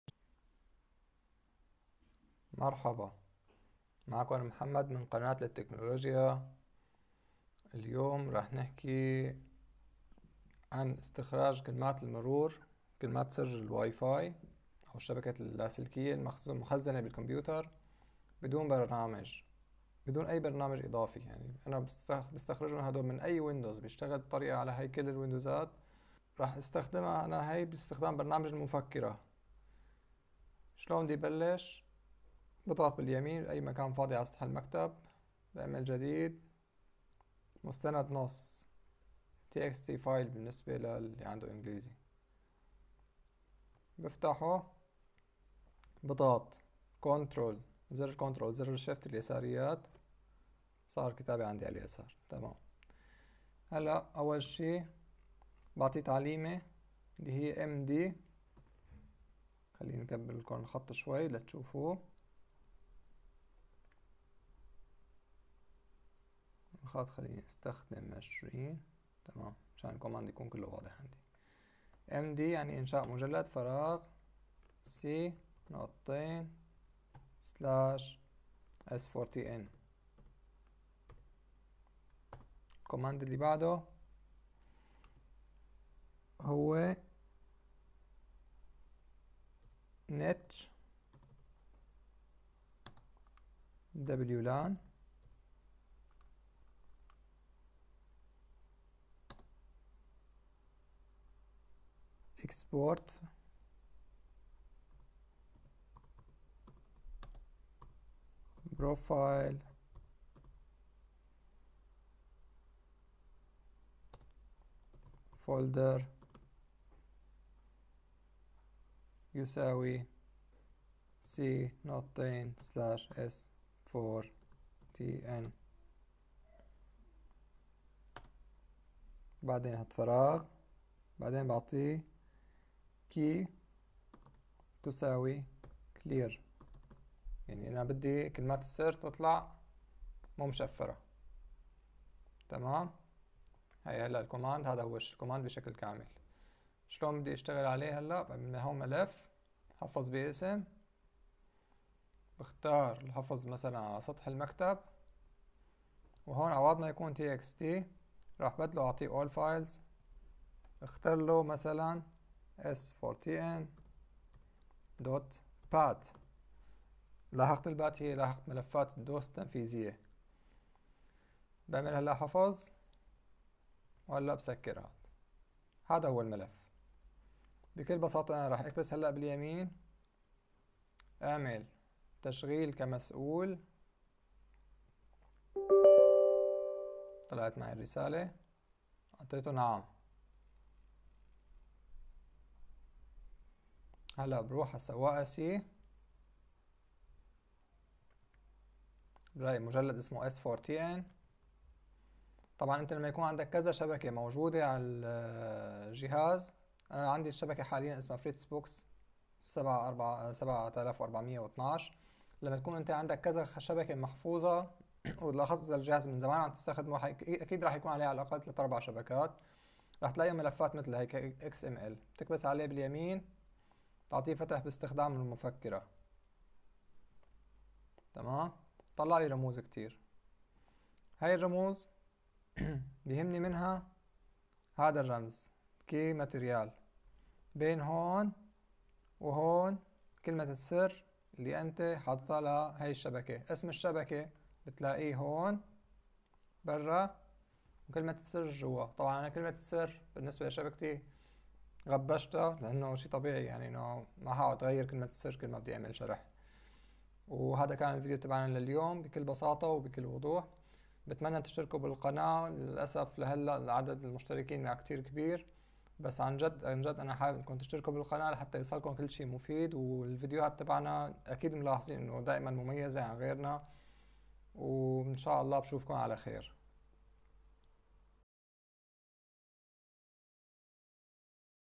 نسيت كلمة الوايرلس ؟ الحل من S4TN بأقل من ثلاث دقائق
فك تشفير كلمات سر الوايرلس , بدون برامج اضافية , فقط باستخدام المفكرة